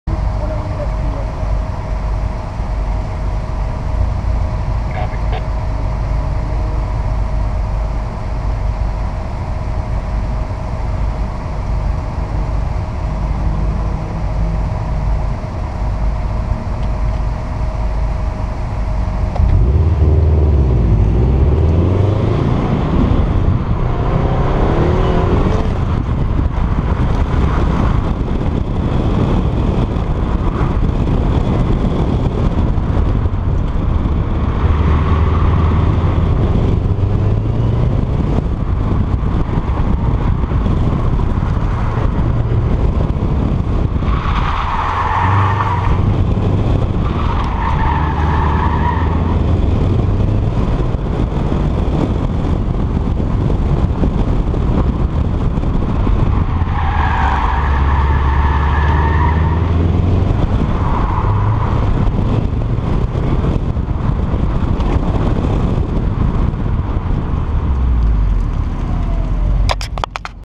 Heat 3 1st Run time = 44.XXX or 45.XXX seconds
2013 WRX at NEPA SCCA in Williamsport, PA
Apirl 12, 2014